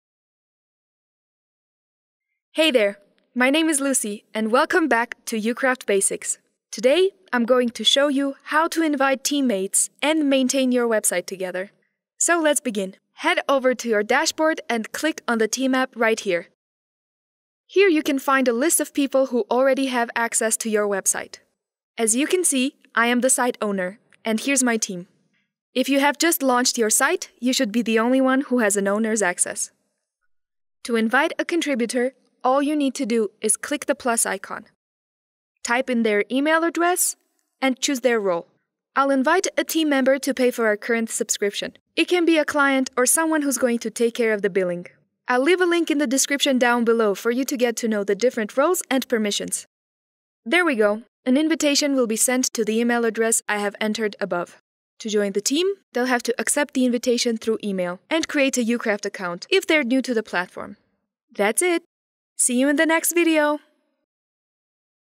Adding People on Board with Ucraft Team App | Basics of Ucraft
In this video, you will learn how to add people to your team for free using the Team App.

Here is how you can invite teammates and maintain your website together! If you head over to your dashboard and click on the Team App you can find a list of people who already have access to your website.
You can invite a team member to pay for our current subscription - it can be a client or someone who’s going to take care of the billing.

To join the team, they’ll have to accept the invitation through email and create a Ucraft account if they’re new to the platform.

That’s it!